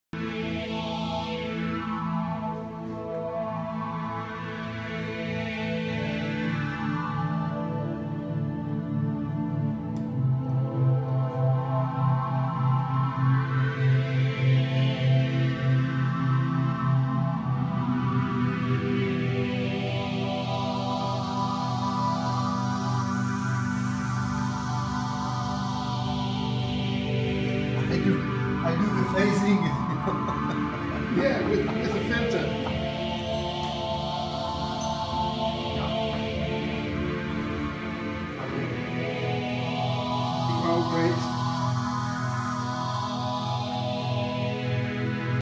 JMJ plays Schmidt
Jean-Michel Jarre plays his Schmidt-Synthesizer. He uses one of the Dual-Filters for a phasing effect.